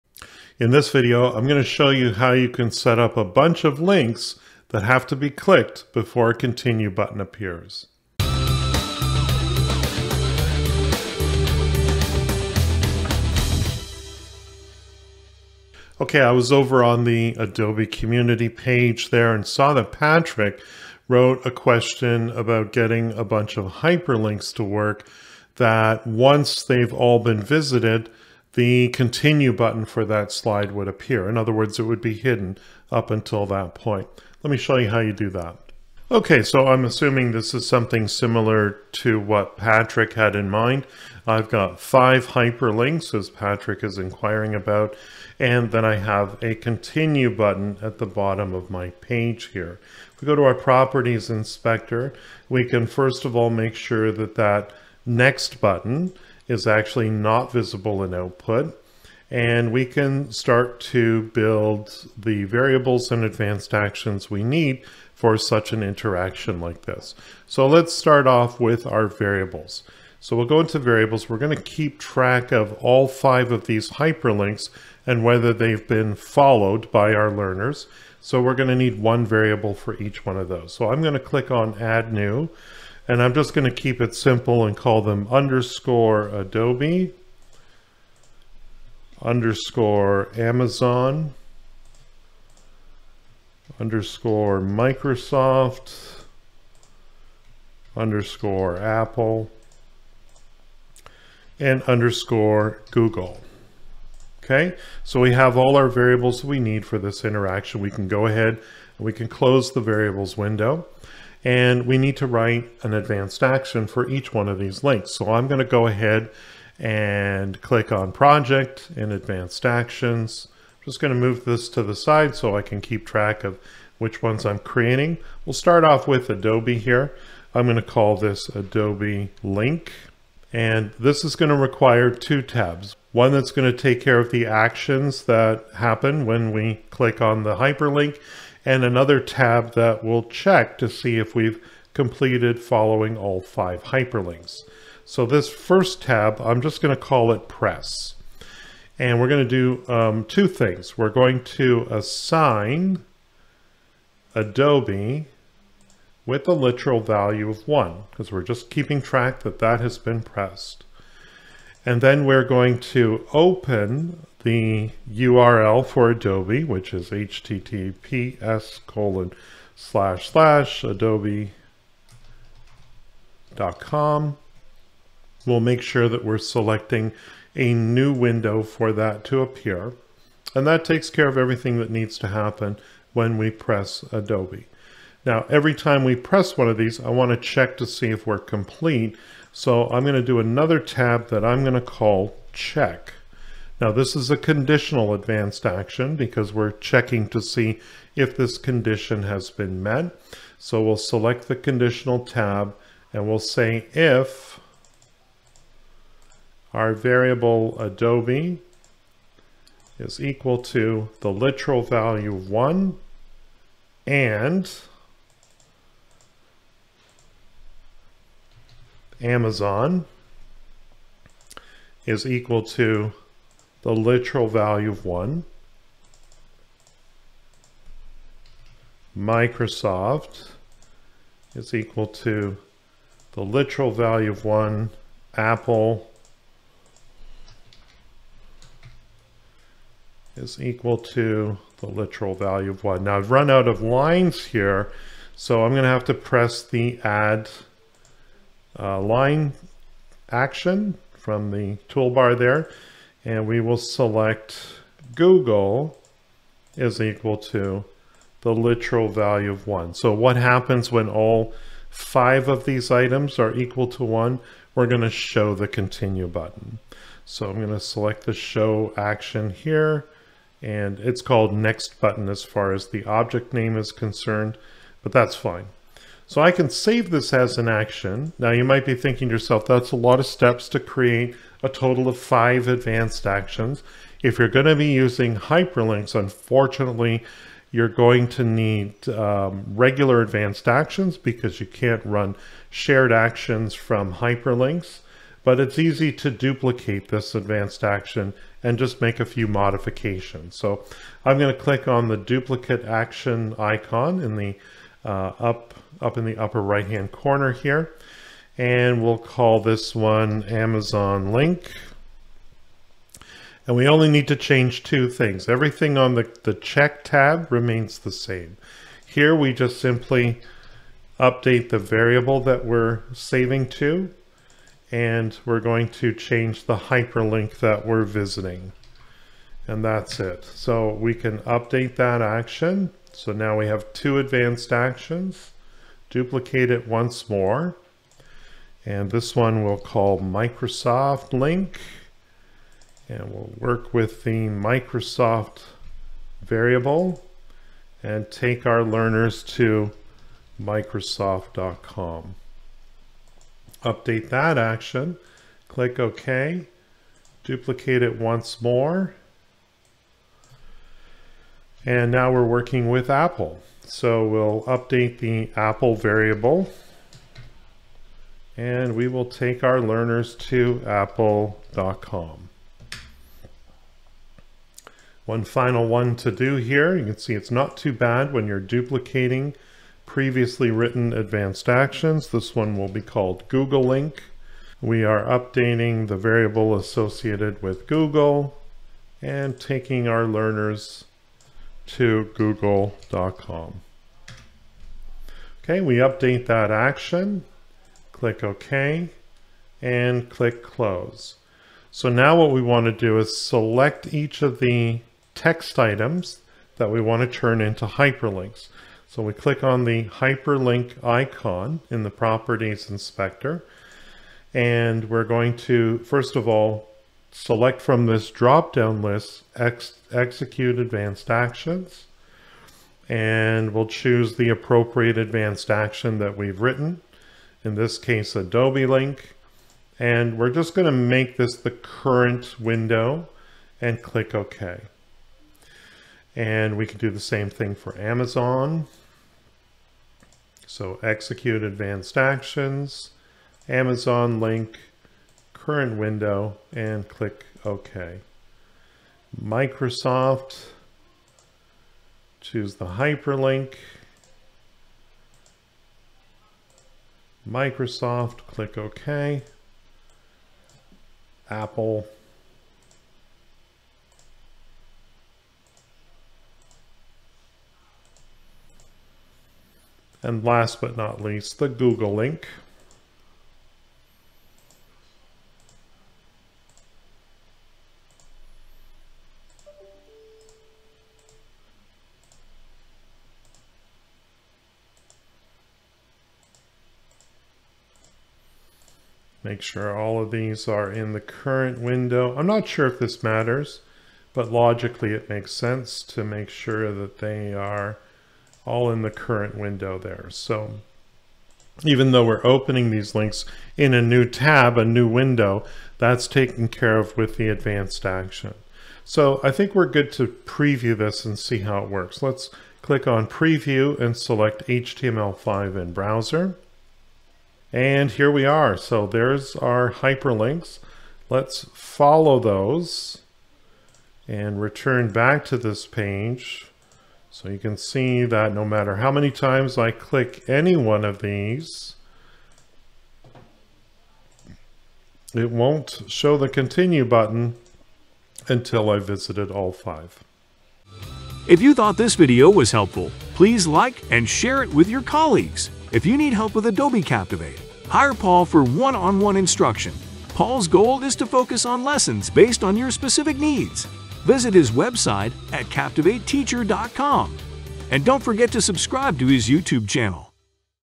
Click All Links Before Next Button Appears in Adobe Captivate 2019
In this video, I will show you how you can require learners to click all links before the next button appears in your Adobe Captivate 2019 slides.